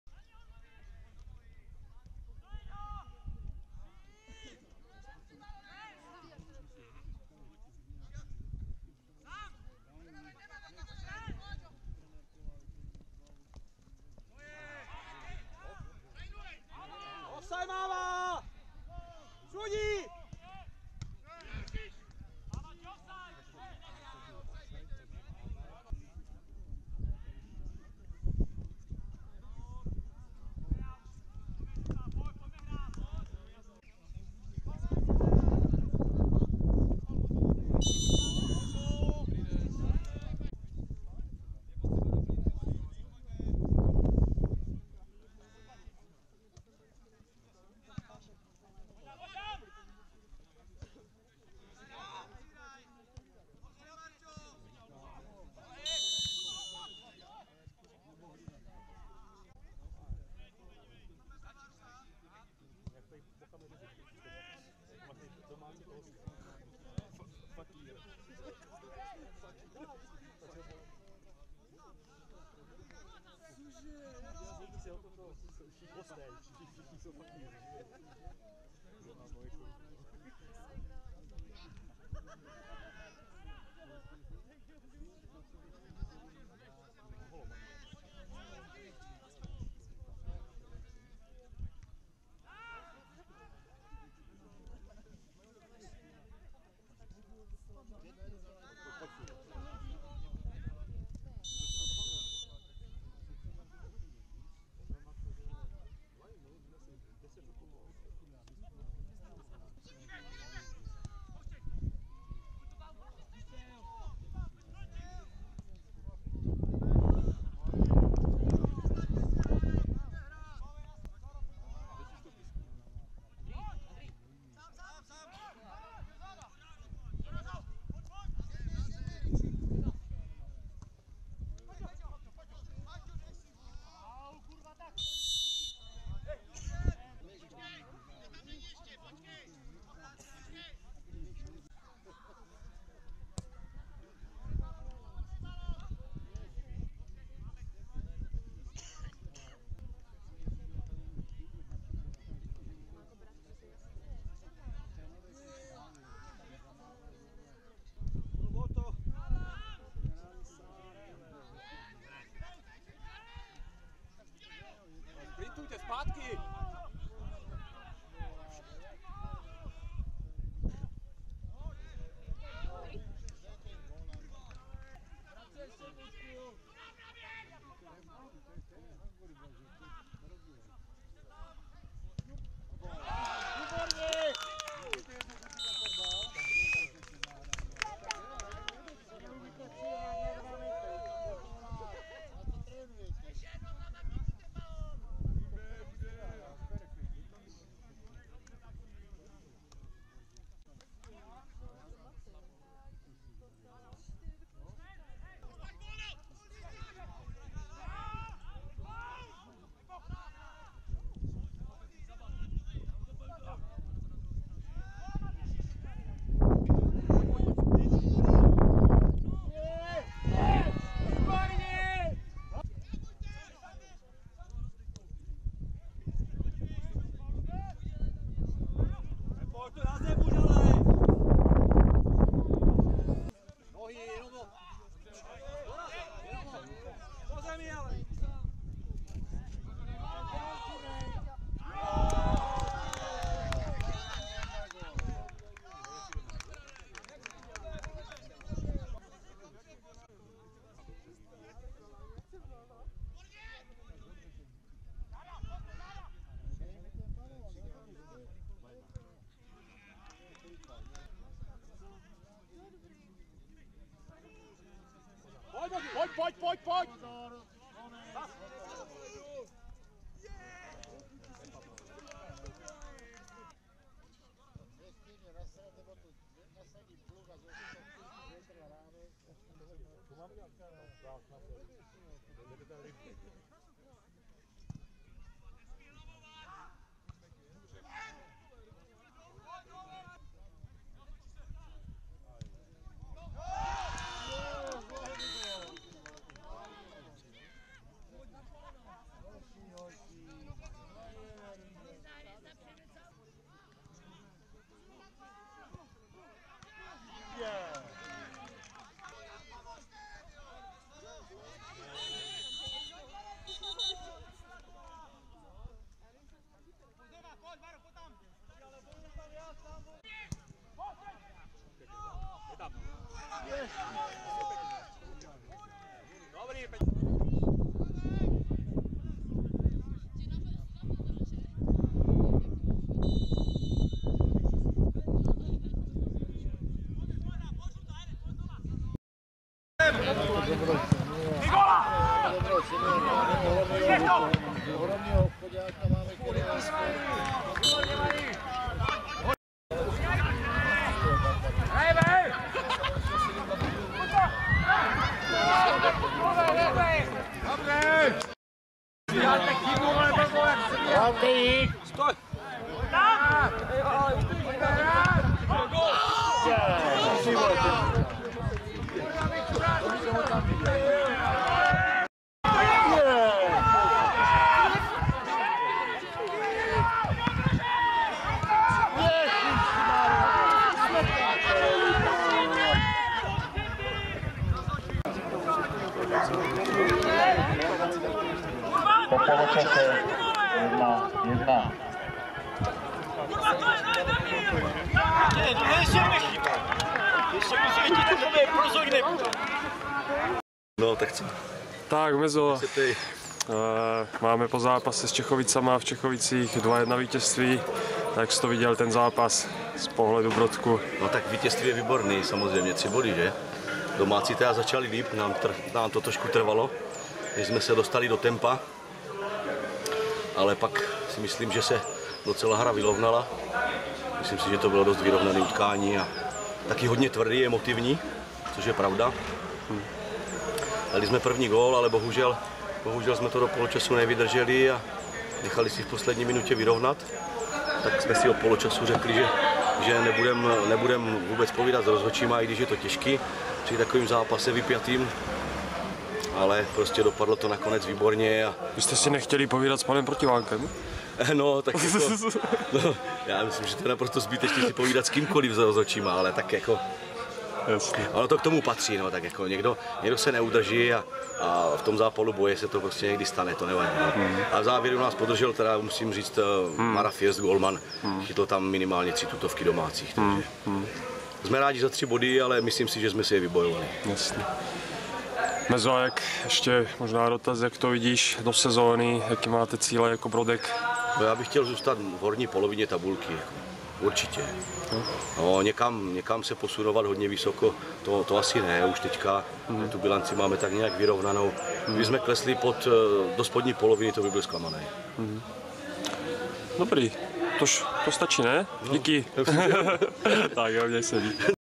Sokol Čechovice B - Brodek u Prostějova 1 : 2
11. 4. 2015 - druhé kolo okresního přeboru Prostějov